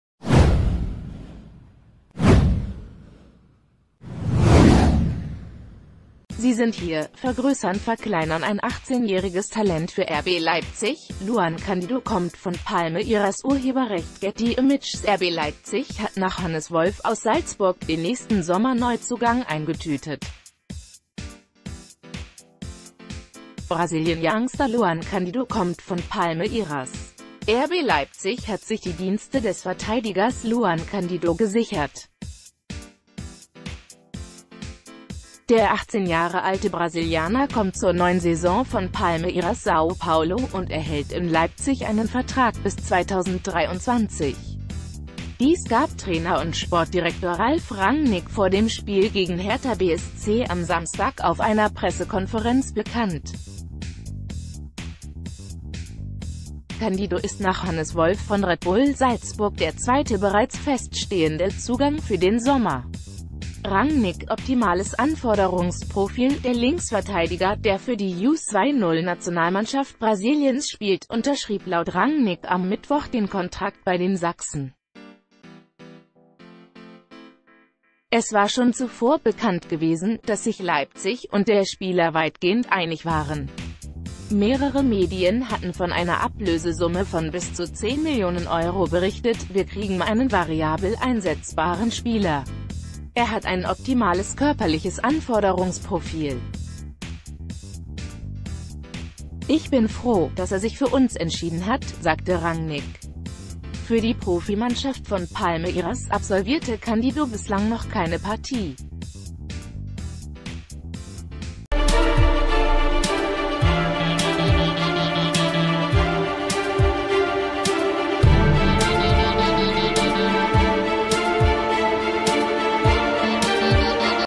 RB Leipzig verpflichtet Luan Candido von Palmeiras
Sie sind hier: vergrößernverkleinern Ein 18-jähriges Talent für RB Leipzig: Luan Candido kommt von Palmeiras © Getty Images RB Leipzig hat nach Hannes Wolf aus Salzburg den nächsten Sommer-Neuzugang eingetütet. Brasilien-Youngster Luan Candido kommt von Palmeiras. RB Leipzig hat sich die Dienste des Verteidigers Luan Candido gesichert. Der 18 Jahre alte Brasilianer kommt zur neuen Saison von Palmeiras Sao Paulo und erhält in Leipzig einen Vertrag bis 2023. Dies gab Trainer und Sportdirektor Ralf...